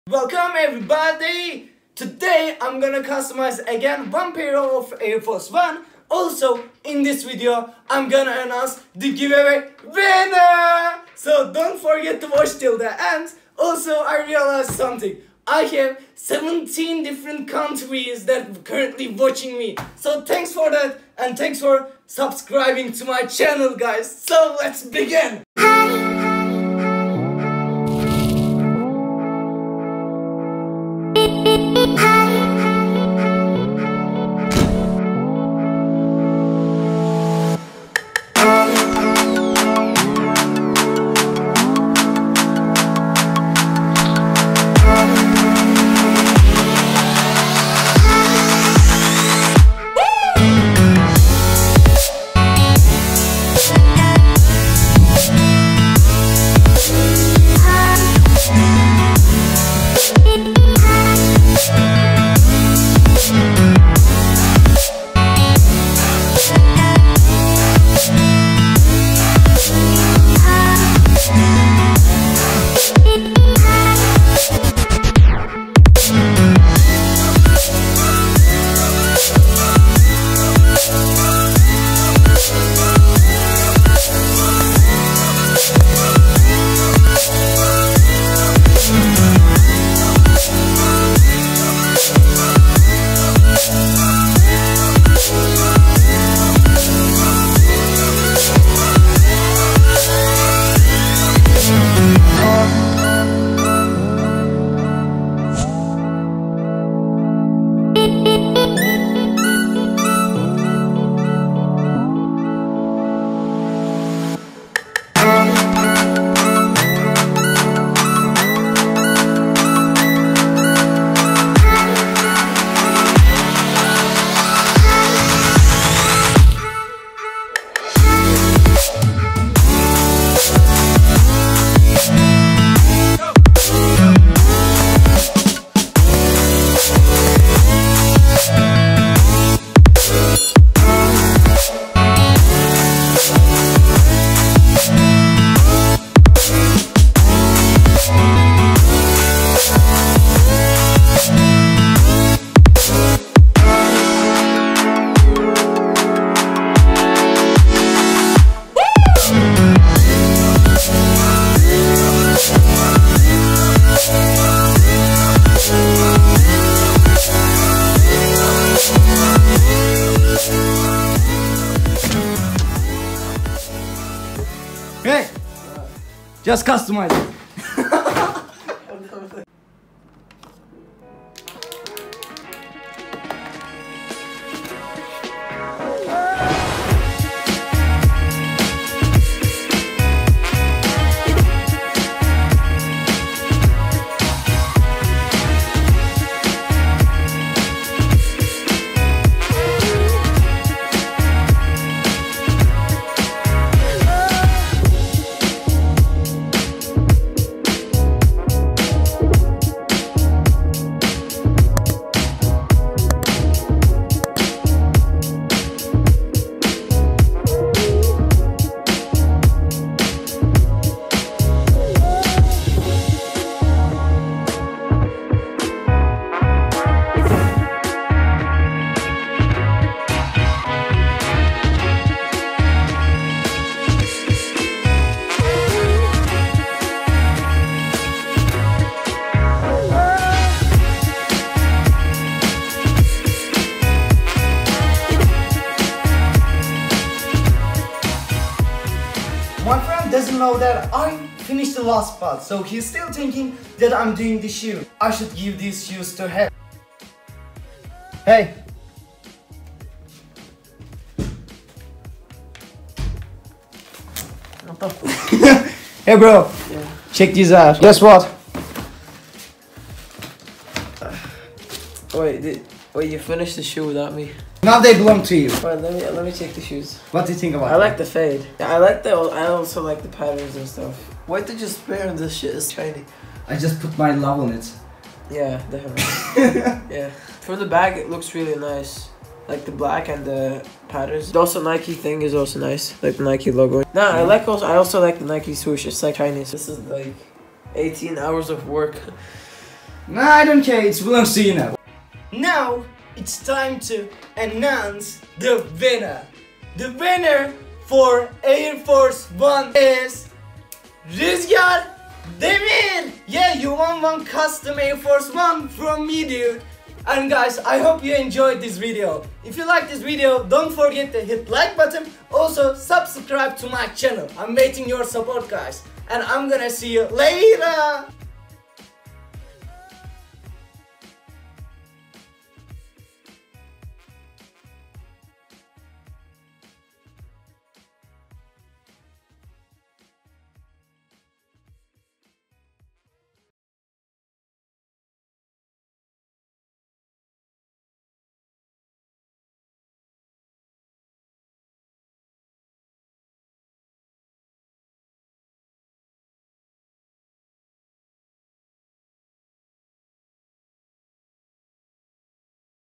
Custom NIKE AIR FORCE1 👟🎨 [GIVEAWAY] as Custom SHOES
Thanks for your support reaching +200 subscribers and more than 500 views at my previous video.

Now It's time to share the winner of the NIKE Air Force 1 GIVEAWAY. To learn the winner don't forget to watch the video. In my next video on the 5th of July 2020 , I'll share the custom shoes [Nike AF1] with some POSCA paint pens for the WINNER of the giveaway. May be Louis Vuitton or Supreme Nike in the next videos. :)

I really want to see your comments about my customization.If you guys like these types of videos, I will make some more custom shoes for a giveaway!

See you at next videos...